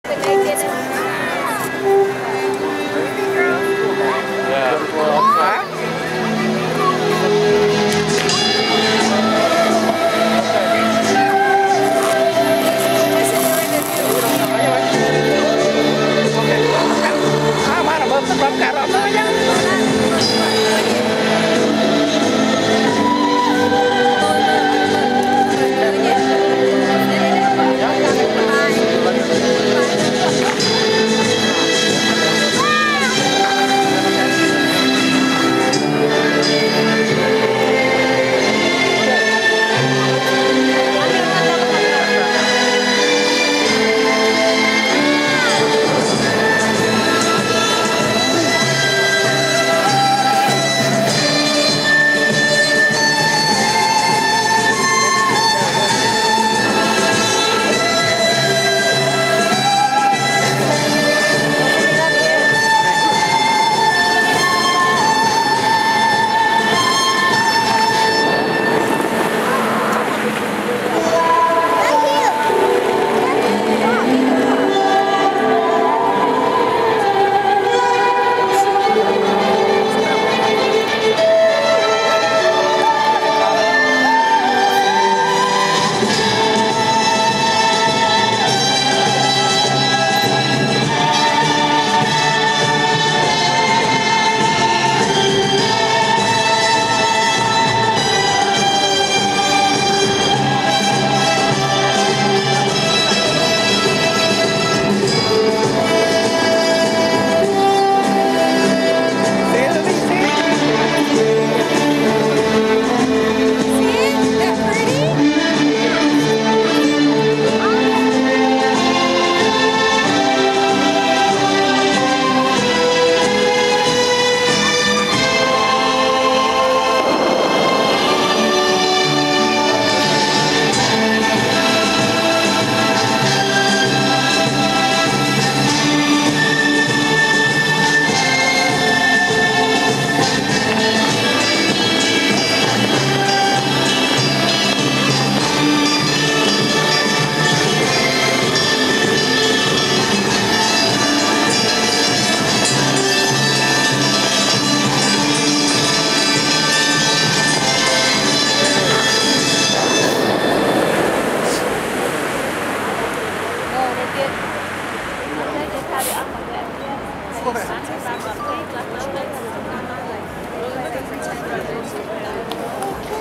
Bellagio Water Show
Bellagio in Las Vegas, NV. 01/02/2011. I thought this would be interesting to video record. Hit me on twitter @YouKnowKennyLo